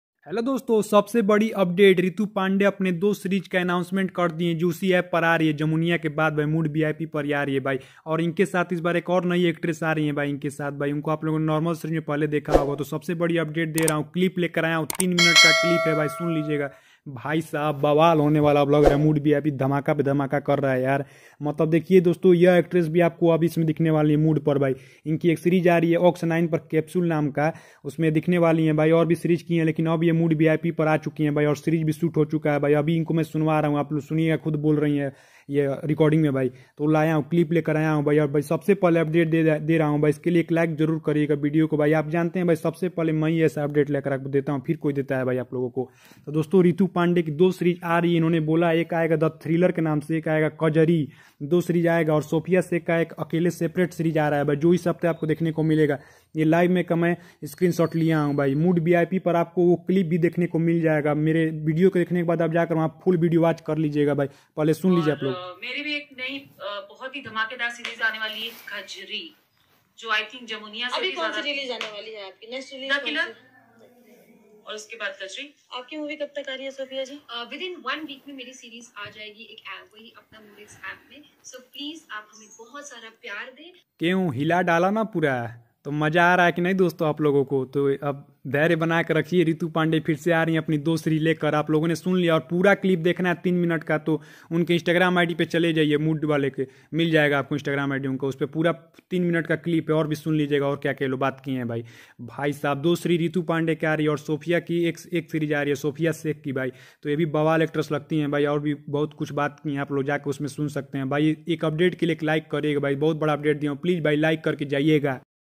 Kajri poster review!/ Ritu Pandey two new upcoming series update/
Kajri poster review! mood vip / Ritu Pandey two new upcoming series update/

Credit Mood vip

Note all image used in this video belong to respected owners//